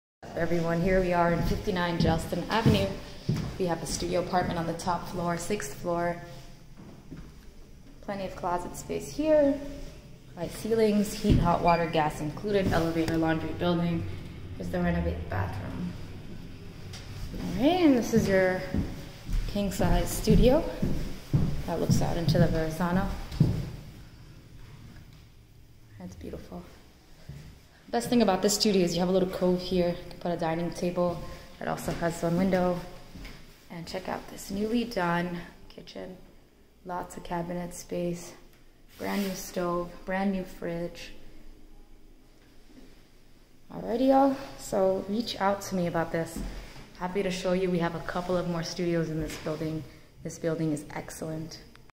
BROOKLYN Apartment TOUR Studio NYC: Separate kitchen, ELEVATOR LAUNDRY for RENT $1350 RENTED
STUDIO with a Separate Kitchen. and. NO FEE!.
EXCELLENT Building. Elevator Laundry.
Steps to R line / along Subway. View of Verrazano Bridge.
Noise proof and clean building. Credit and income check fee by management.  Price negotiable.

or find my cell via my streeteasy